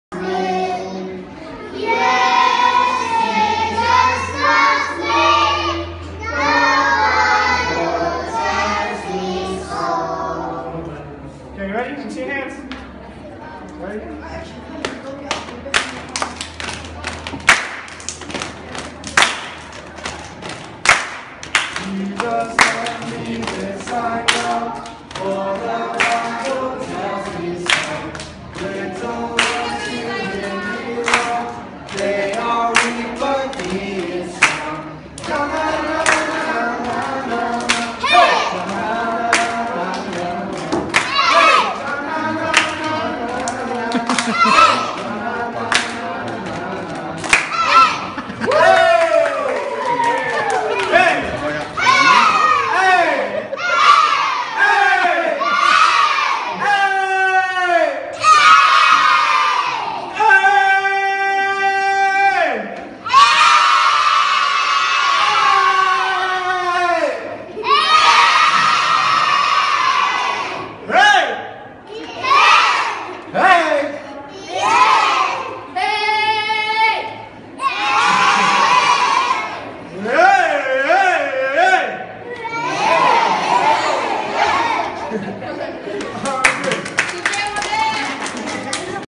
SoCal Youth with Beginners Deptt. Chaltlang Young People Corps
9th July 2017 Sunday School at Young People Corps, Chaltlang..SoCal Youth interacting with the Cradle Roll - Beginners Department